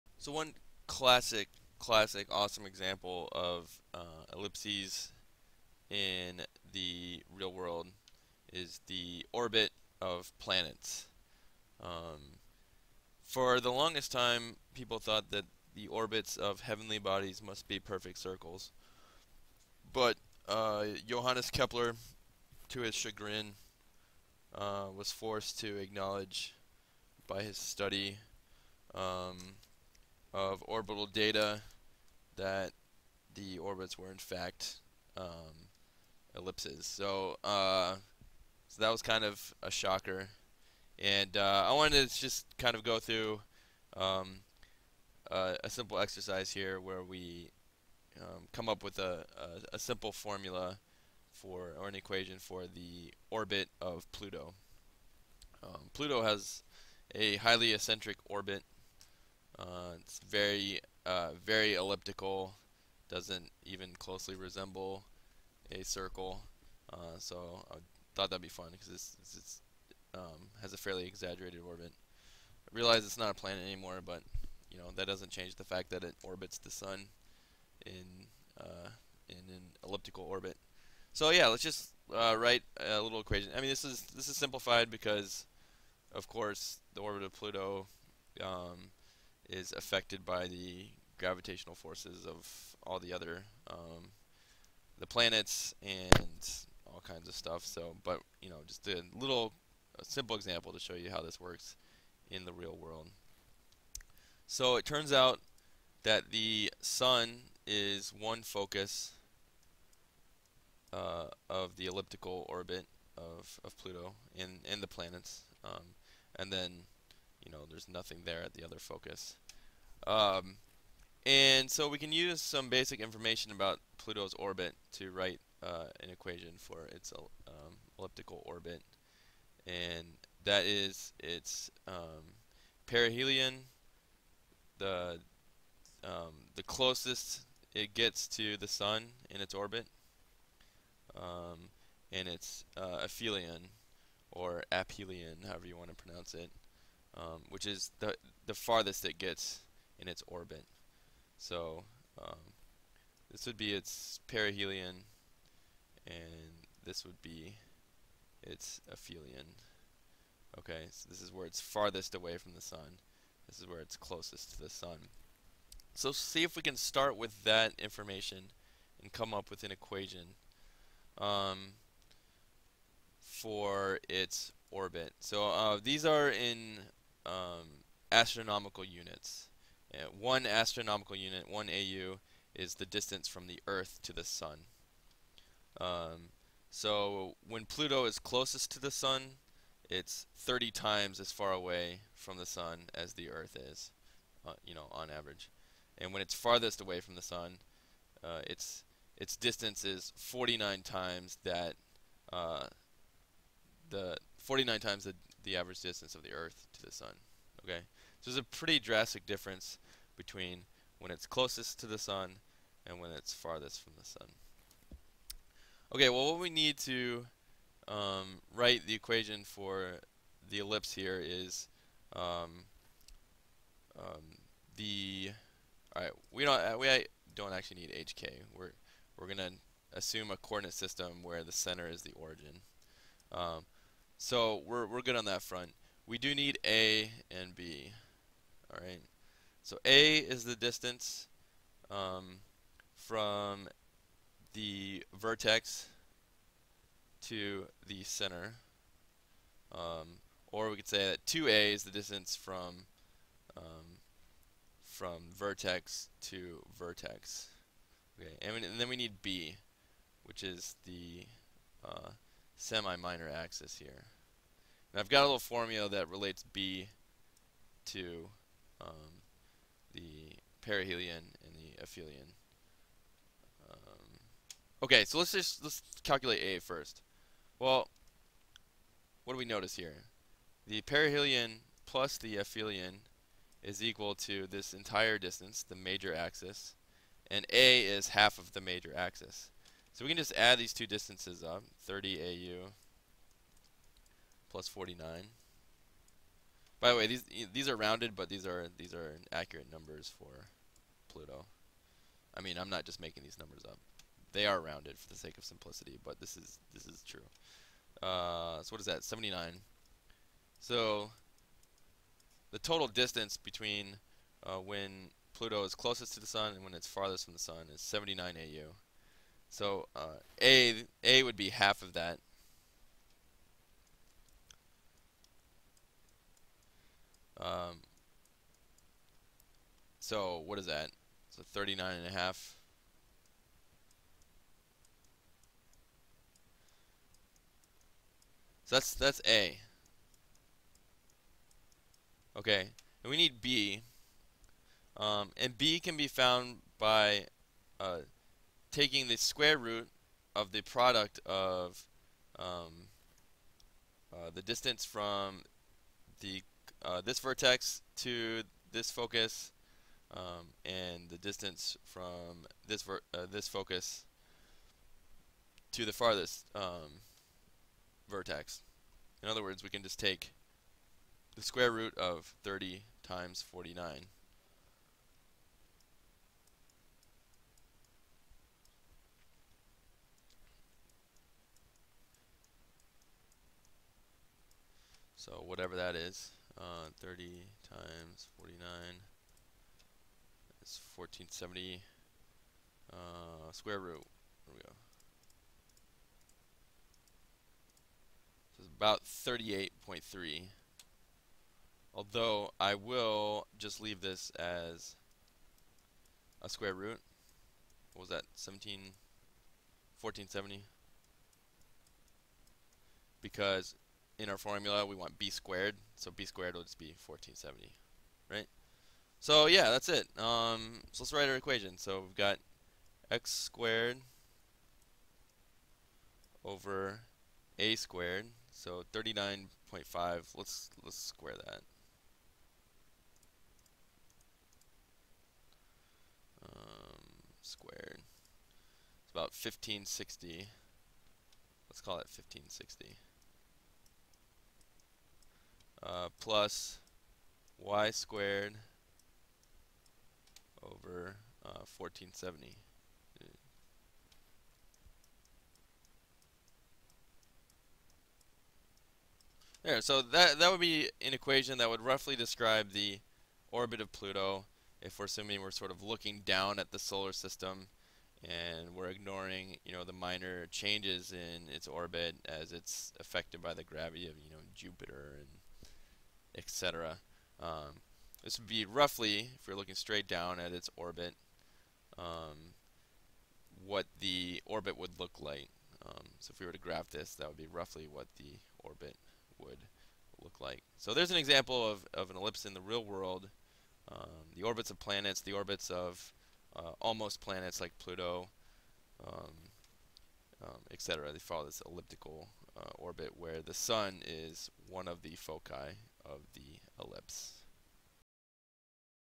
Ellipses in the Real World
| In this lesson, we'll use some basic information about Pluto's elliptical orbit around the sun to write an equation to (very roughly) describe the path it takes through our solar system!